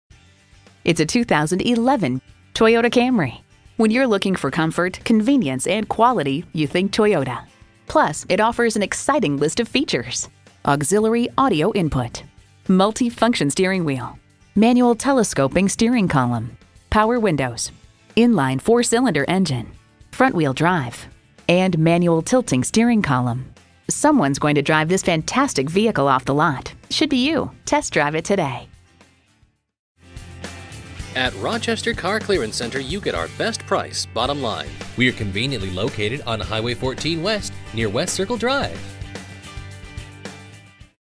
2011 Toyota Camry Rochester Winona, MN XA10395 - SOLD
Year: 2011
Make: Toyota
Model: Camry
Trim: LE
Engine: 2.5L 4-Cylinder SMPI DOHC
Transmission: 6-Speed Automatic
Color: Green
Interior: Dark Charcoal
Mileage: 273126
Stock #: XA10395
VIN: 4T4BF3EK9BR138734
2011 Green Toyota Camry LE This Toyota Camry has many features and is well equipped including.brbrPriced below KBB Fair Purchase Price! Clean CARFAX.brbrThank you for taking the time to look at this great 2011 Toyota Camry. We look forward to earning your business!

Tune-Ups
Transmission Service
Brake Service
Performance Parts
Factory Wheels

Address:
2980 HWY 14